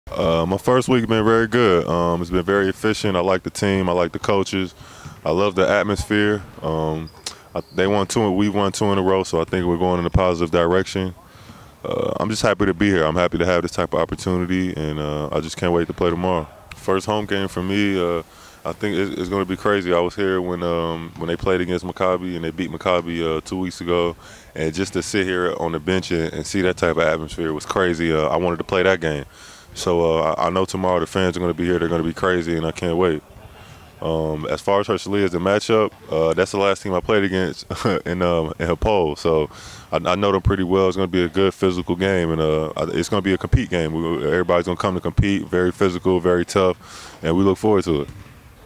האוול מסכם שבוע ראשון בהפועל ומתכונן למפגש מול הרצליה
ריצ'ארד האוול מסכם שבוע ראשון בהפועל ומחכה לפגוש אתכם מחר (21:00) בארנה!

Richard Howell sums up his 1st week with Hapoel Jerusalem and looking forward for his 1st home match tomorrow against Bnei Herzliya